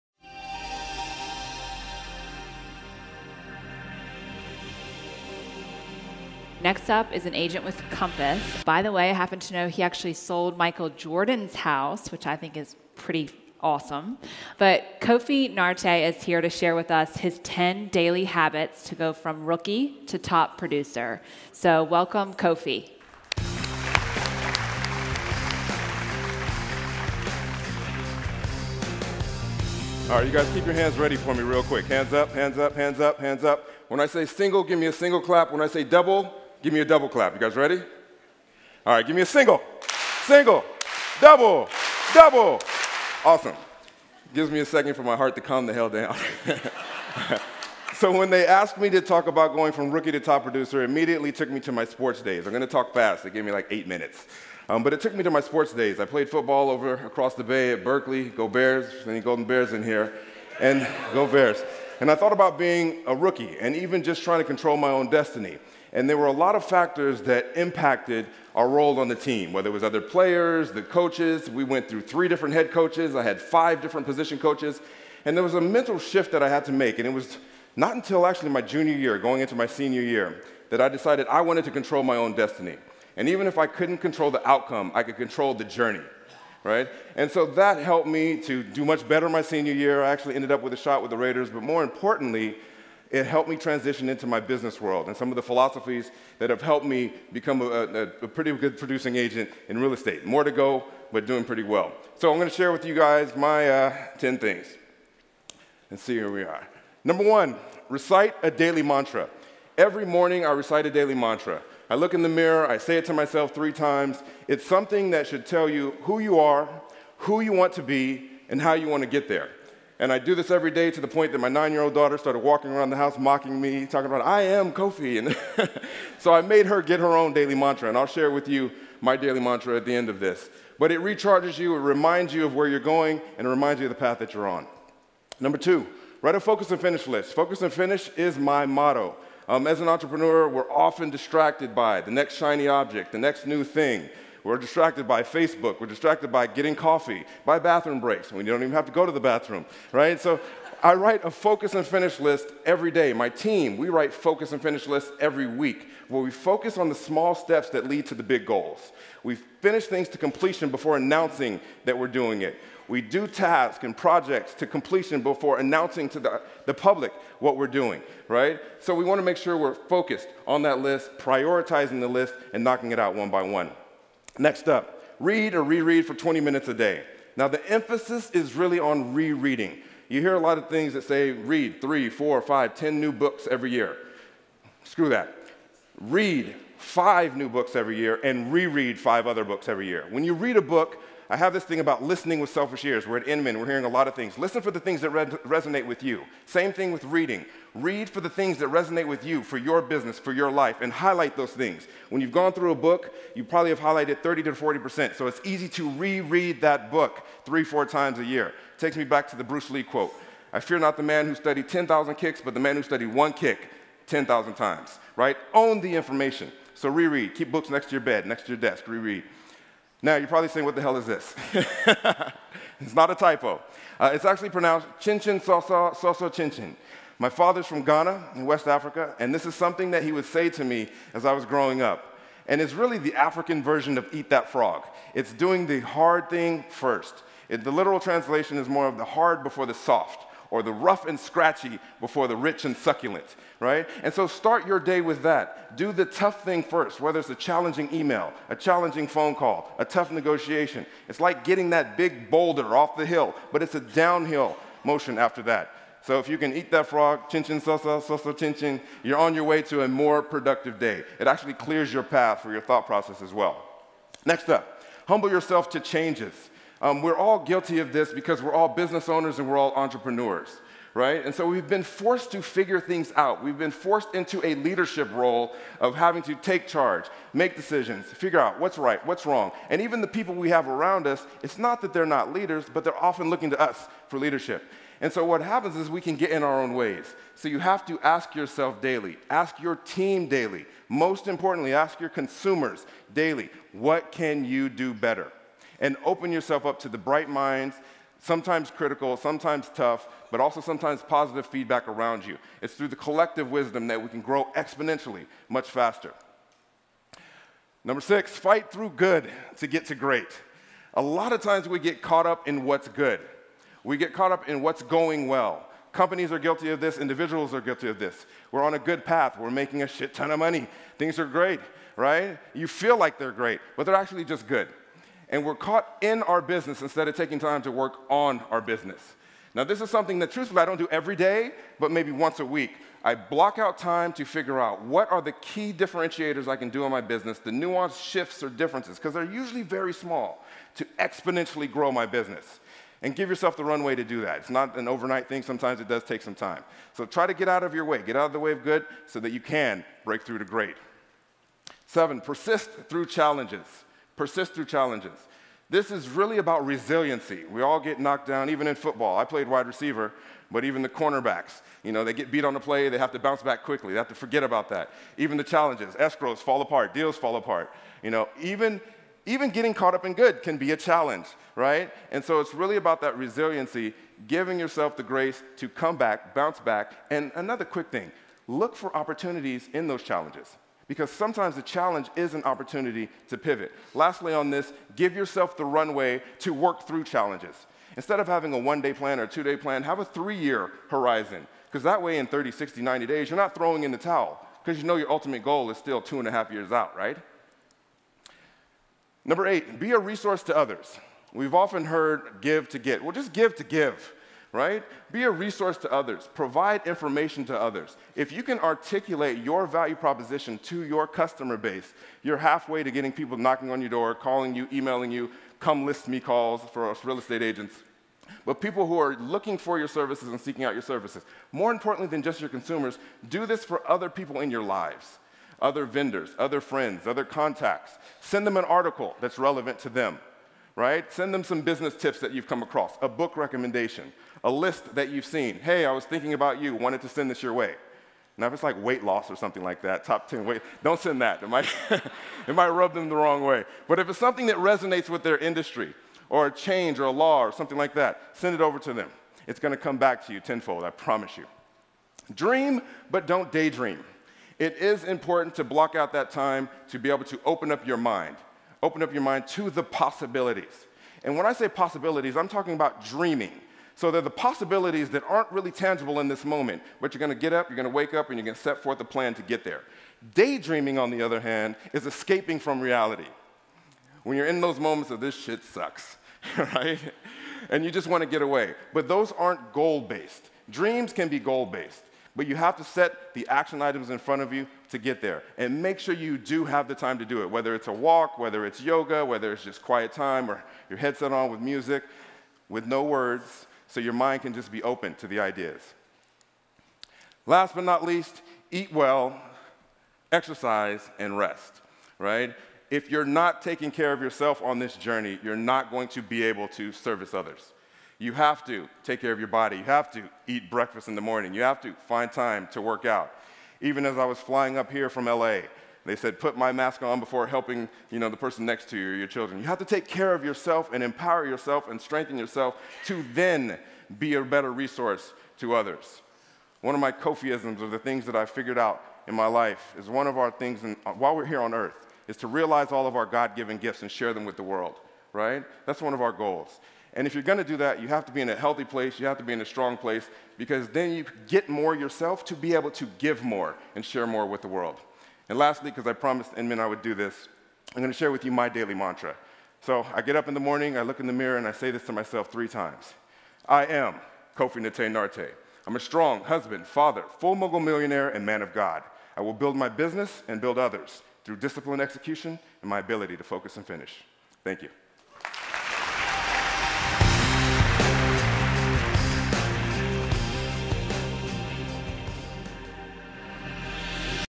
10 Daily Success Habits - Kofi Nartey
MOTIVATION: Check out this power packed talk I gave a couple years ago to about 4000 entrepreneurs in the room at Inman Connect and over 100k people online. Your habits determine your outcome. Focus on the disciplined journey and not the outcome. Success habits. Motivation. Goals. Focus & Finish! Let's get after it! FullMogul -Kofi Nartey